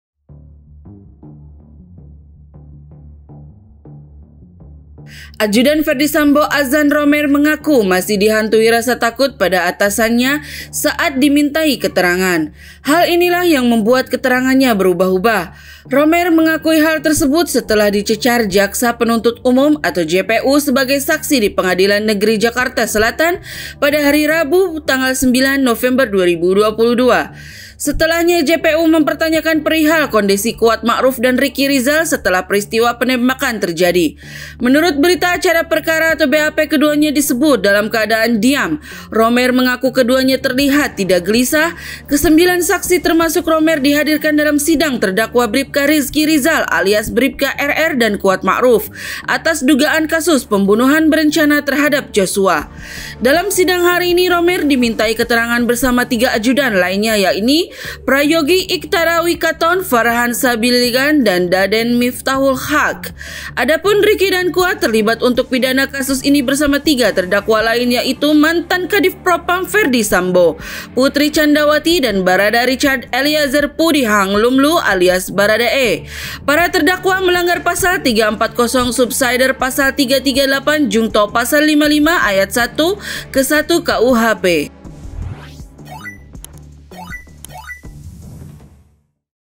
Ajudan Akui Takut Kepada Ferdy Sambo berita viral
Prabunews - Ajudan Ferdy Sambo, Adzan Romer mengaku masih dihantui rasa takut pada atasannya saat dimintai keterangan. Hal ini lah yang membuat keterangannya berubah-ubah.

Romer mengakui hal tersebut setalah dicecar Jaksa Penuntut Umum (JPU) sebagai saksi di Pengadilan Negeri Jakarta Selatan, Rabu (9/11).

"Apa yang menyebabkan keterangan saudara berubah-ubah?" tanya Jaksa.

"Karena awalnya kami masih takut memberikan kejujuran," jawab Romer.

"Takut memberikan kejujuran, takut kepada siapa? Kita kan takut pada Tuhan, kita takut mati, atau kita takut apa?" desak Jaksa.

"Takut sama Bapak, Pak," sahut Romer.

"Bapak siapa?" tanya Jaksa.

"Pak Sambo," jelasnya.

"Jadi takut dengan Ferdy Sambo?" Jaksa mengulang.

"Iya," tegas Romer.

Setelahnga, JPU mempertanyakan perihal kondisi Kuat Ma'ruf dan Ricky Rizal setelah peristiwa penembakan terjadi.

Menurut Berita Acara Perkara (BAP), keduanya disebut dalam keadaan diam. Romer mengaku keduanya terlihat tidak gelisah.

"Tidak kah saudara melihat adanya kegelisahan dari terdakwa RR dan KM?" tanya Jaksa.

"Ketika saya tanya tidak dijawab," jawab Romer.

"Tidak ada kepanikan?" tanya Jaksa lagi.

"Iya, betul," tegasnya.

Kesembilan saksi, termasuk Romer dihadirkan dalam sidang terdakwa Bripka Ricky Rizal alias Bripka RR dan Kuat Ma'ruf atas dugaan kasus pembunuhan berencana terhadap Yosua.

Dalam sidang hari ini, Romer dimintai keterangan bersama tiga ajudan lainnya. Yakni Prayogi Iktara Wikaton, Farhan Sabilillag, dan Daden Miftahul Haq.